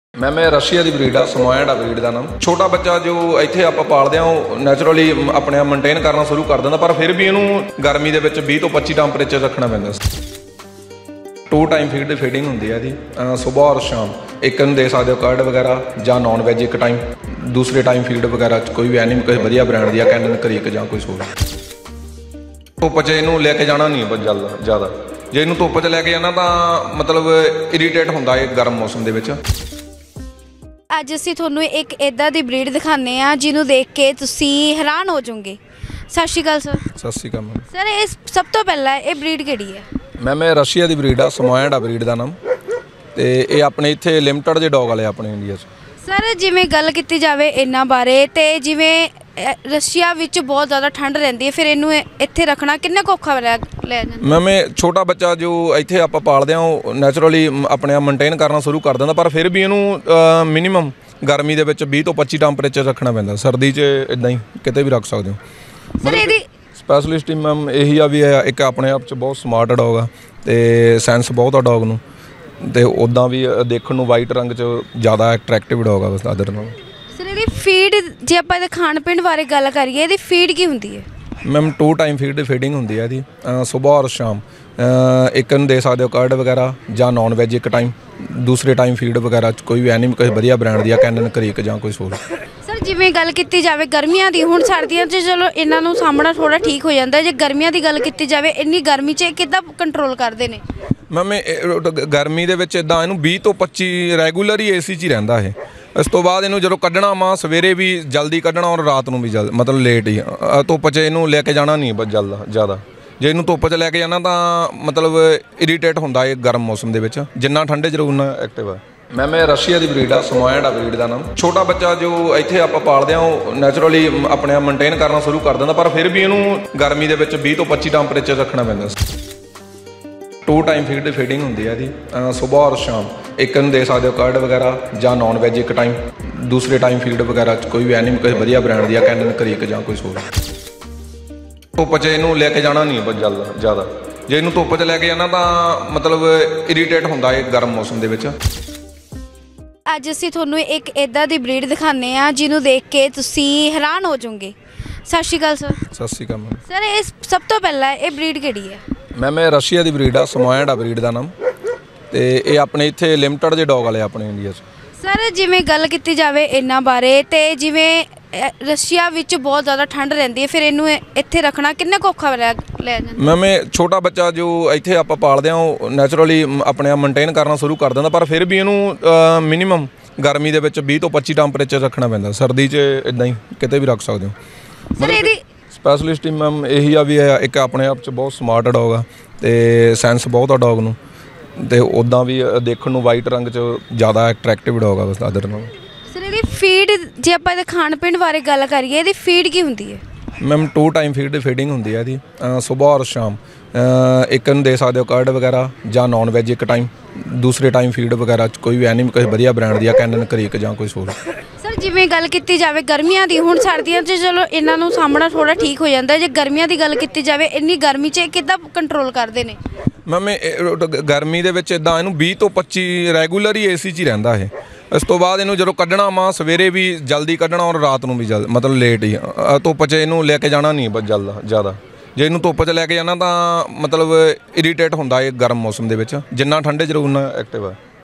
ਅਮੀਰਾਂ ਵਾਲੇ ਸ਼ੌਂਕ ਨੇ ਇਸ Russian Dog ਦੇ! ਗਰਮੀਆਂ 'ਚ ਬਿਨਾਂ AC ਨਹੀਂ ਰਹਿੰਦਾ ਧੁੱਪ ਤੋਂ ਤਾਂ ਹੈ ਇਸਨੂੰ Allergy!
ਅਮੀਰਾਂ ਵਾਲੇ ਸ਼ੌਂਕ ਨੇ ਇਸ Russian Dog ਦੇ!
ਗਰਮੀਆਂ 'ਚ ਬਿਨਾਂ AC ਨਹੀਂ ਰਹਿੰਦਾ,
ਧੁੱਪ ਤੋਂ ਤਾਂ ਹੈ ਇਸਨੂੰ Allergy!